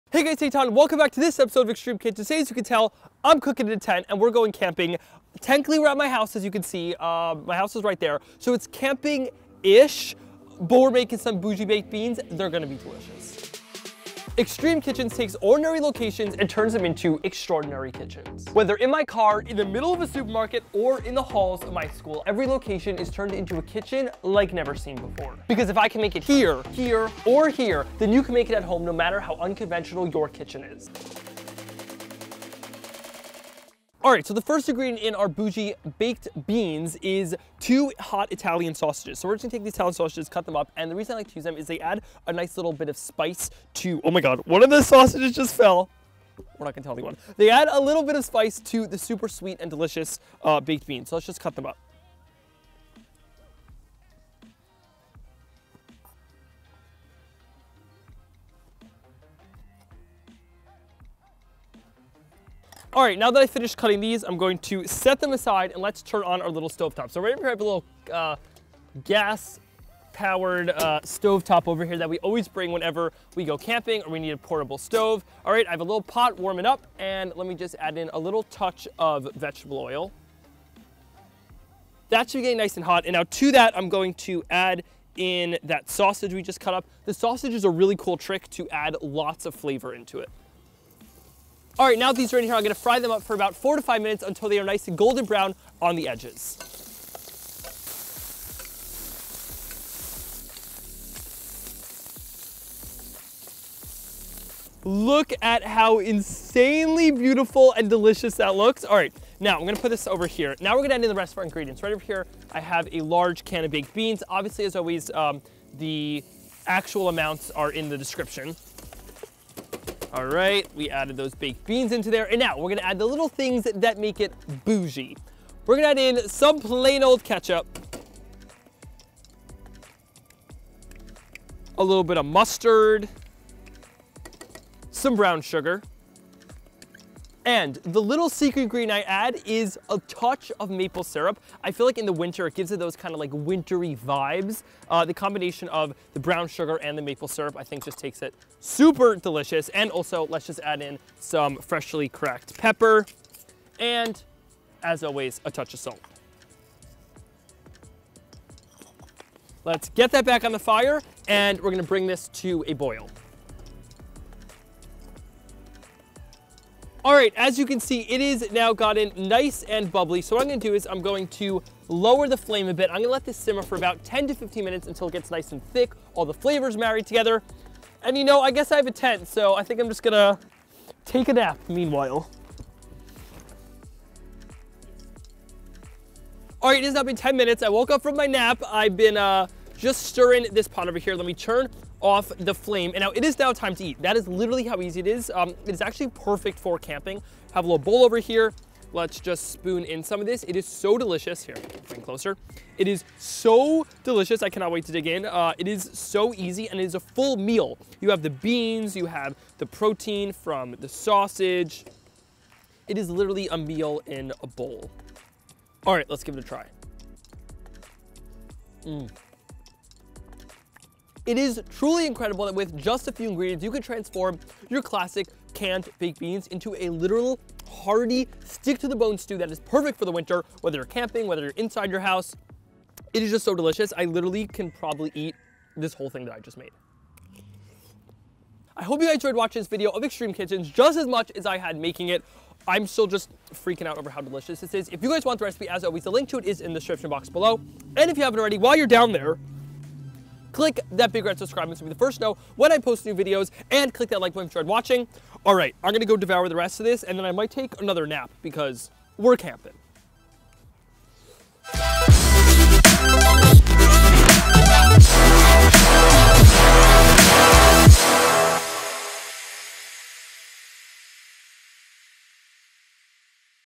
Cooking In A Tent | Extreme Kitchens E1 | Eitan Bernath
Today is the premiere of my brand new show, Extreme Kitchens! In this episode, I am taking the kitchen outside and cooking in a tent! I'm elevating classic canned baked beans and making them a bit more boujee. It's packed with tons of flavor from the spicy Italian sausage, maple syrup, and more! If I can make it in a tent, then you can make it at home no matter how unconventional your kitchen is at home!